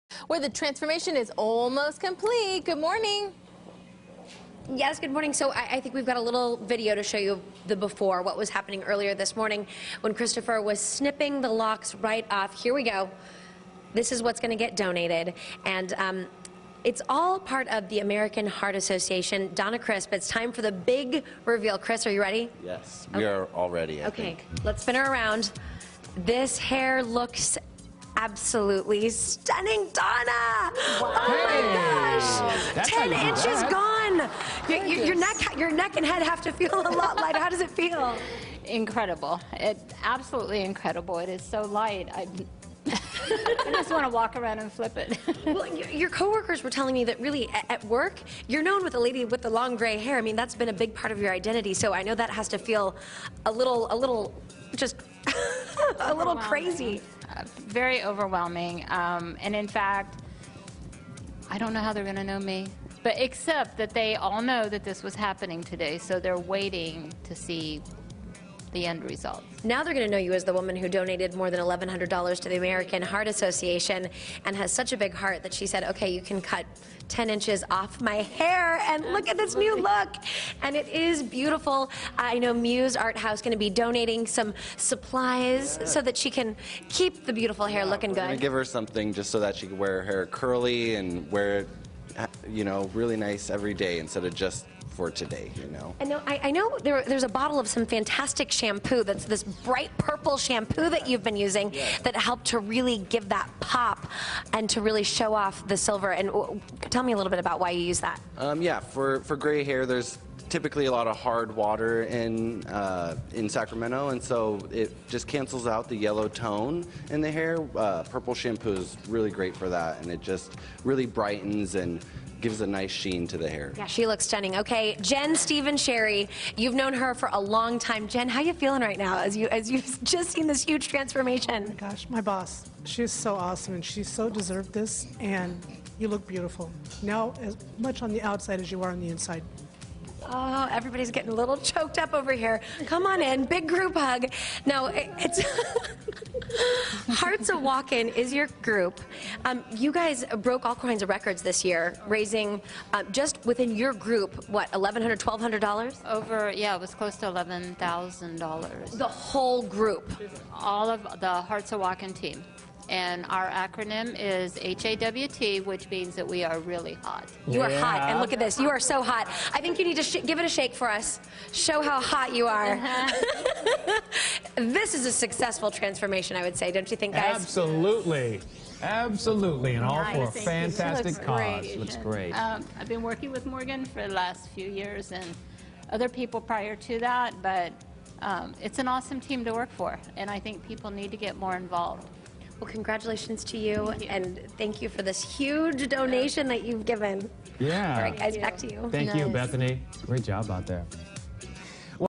10" of Hair for $1,000 Pt. 2
Let’s see how this woman’s hair turned out after donating 10 inches of it to charity.